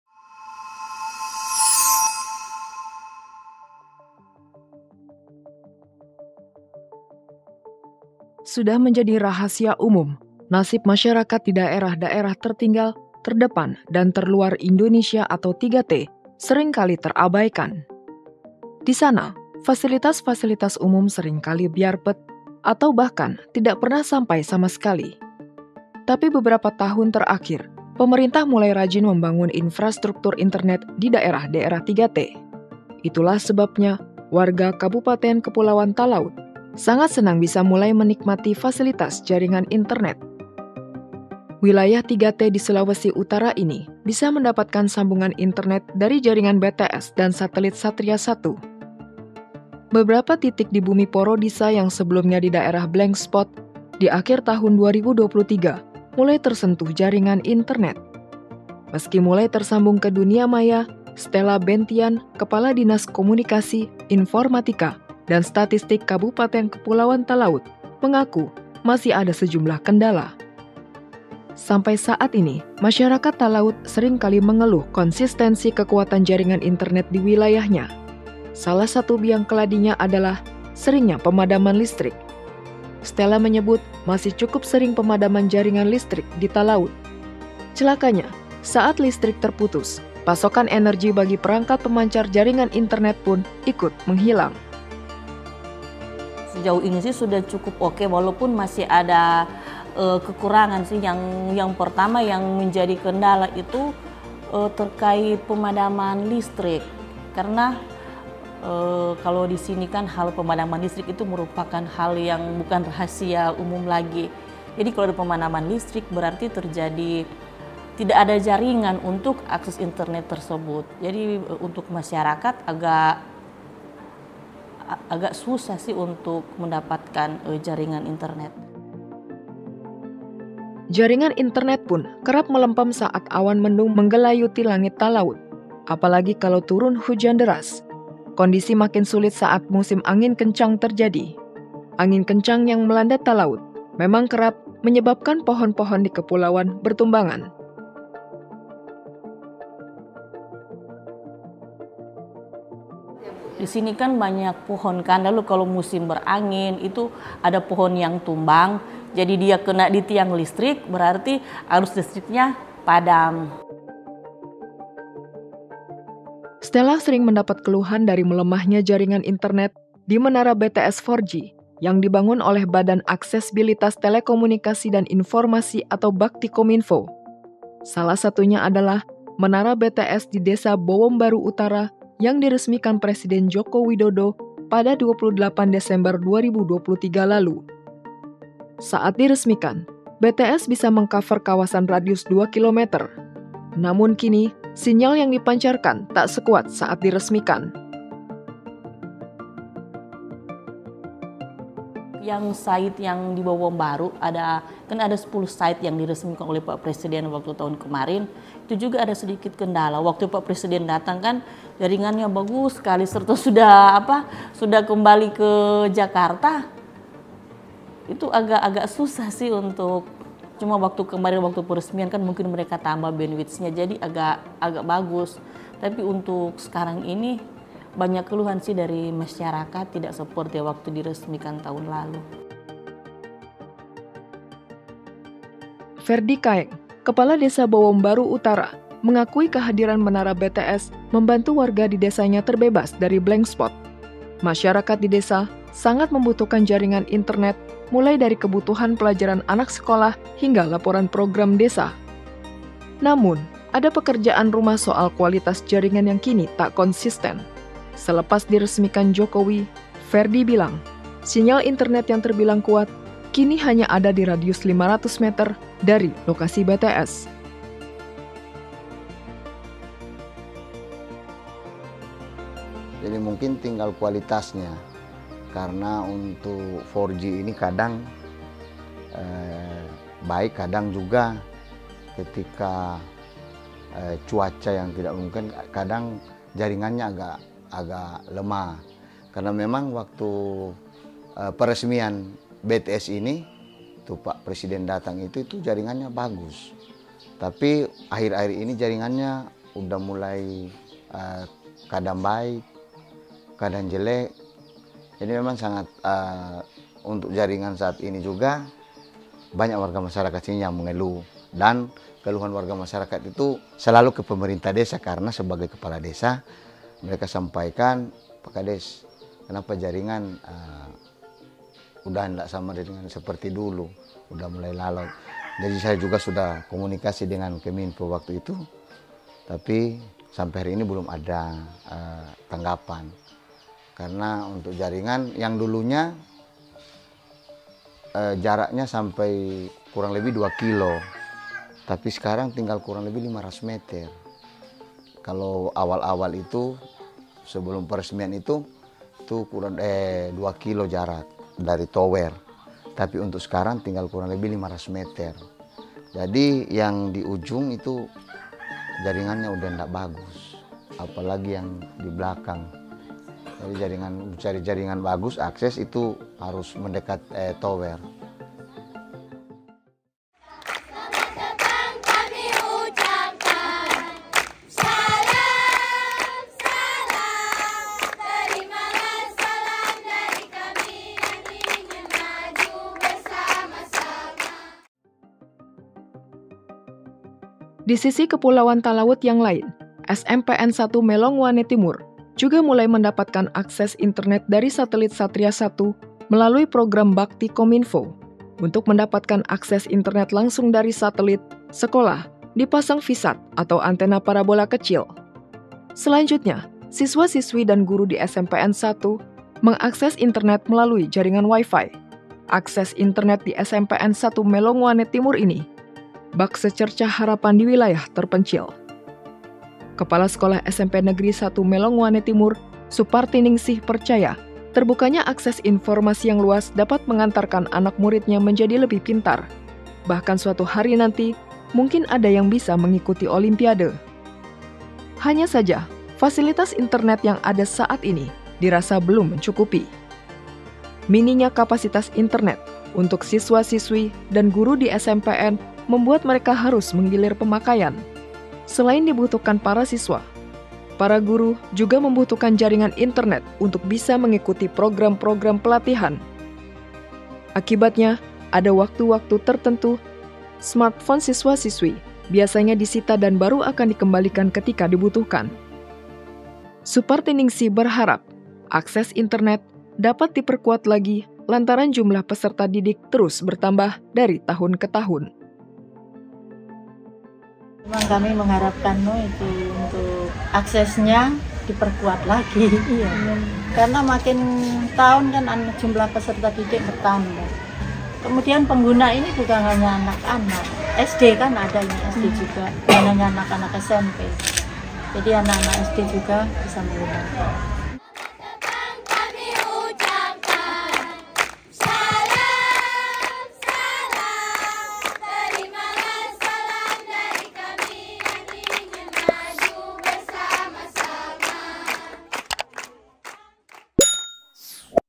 Internet di Talaud, Kawasan 3T Ujung Utara Indonesia I KONTAN Jelajah Infrastruktur Berkelanjutan
Sudah menjadi rahasia umum, nasib masyarakat di daerah-daerah tertinggal, terdepan, dan terluar Indonesia (3T) sering kali terabaikan. Di sana fasilitas-fasilitas umum sering kali byar pet, atau bahkan tidak pernah sampai sama sekali. Tapi beberapa tahun terakhir, pemerintah mulai rajin membangun infrastruktur internet di daerah-daerah 3T.

Itulah sebabnya warga Kabupaten Kepulauan Talaud sangat senang bisa mulai menikmati fasilitas jaringan internet. Wilayah 3T di Sulawesi Utara ini bisa mendapatkan sambungan internet dari jaringan BTS dan satelit Satria-1.

Beberapa titik di Bumi Porodisa yang sebelumnya daerah blank spot, di akhir tahun 2023 mulai tersentuh jaringan internet. Meski mulai tersambung ke dunia maya, Sthela Bentian Kepala Dinas Komunikasi, Informatika dan Statistik Kabupaten Kepulauan Talaud mengaku masih ada sejumlah kendala. Sampai saat ini masyarakat Talaud sering kali mengeluh konsistensi kekuatan jaringan internet di wilayahnya.